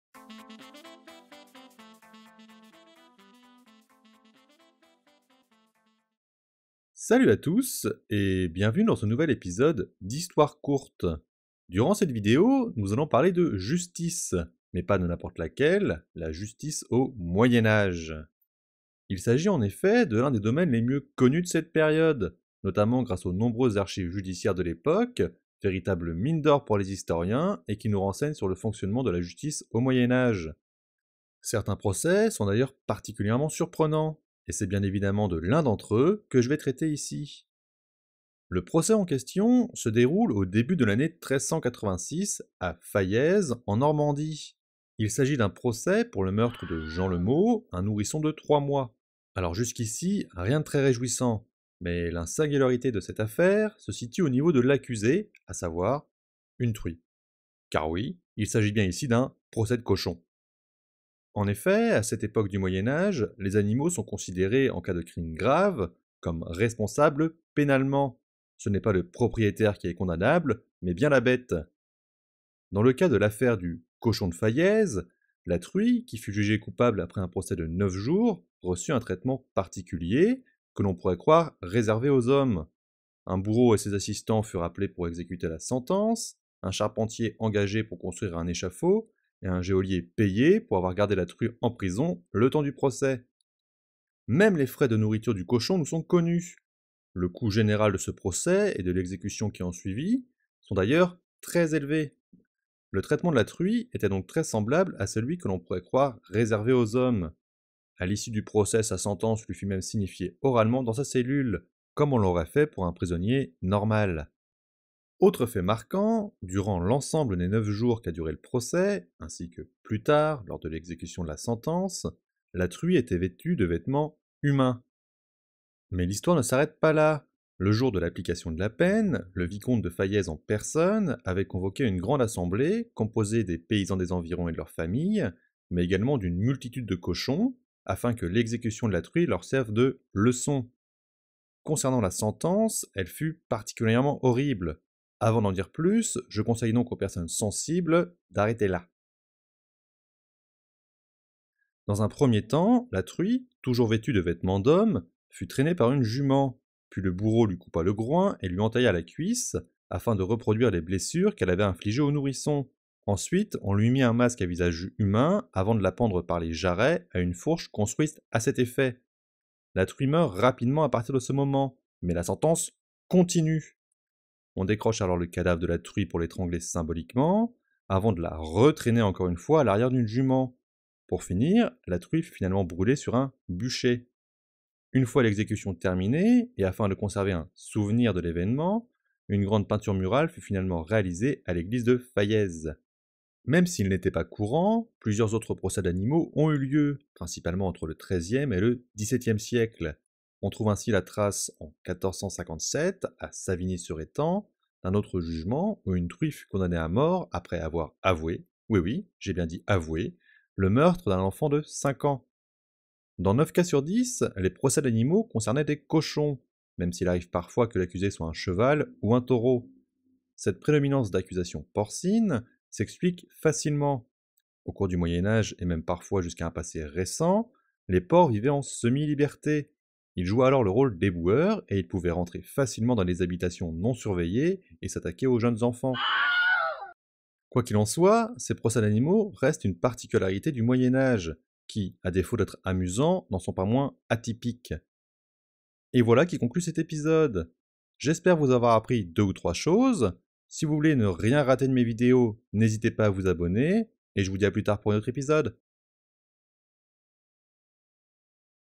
Procès atypique du Moyen-Âge : Une affaire de cochon.
L'histoire d'un procès peu banal du Moyen-Âge : une affaire de cochon !